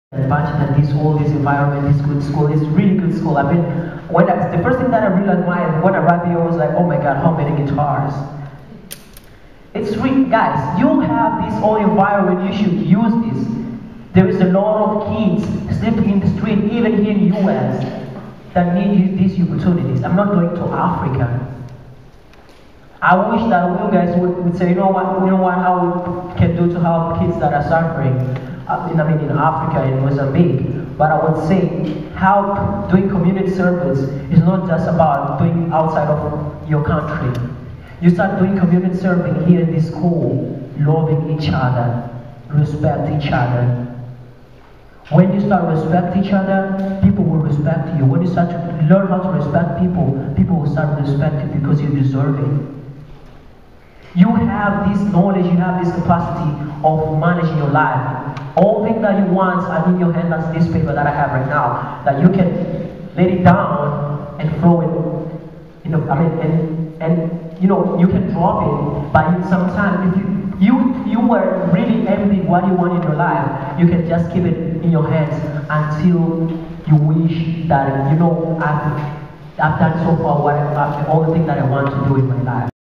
Alcides @ Locke: "Community Service"
Alcides Soares, of Mozambique Africa, speaks at Locke School about life in Africa, how AIDS has effected his community, and the idea of community service, both here in Chicago and in Africa.

Check out his movie "Home is Where You Find It" for true inspiration!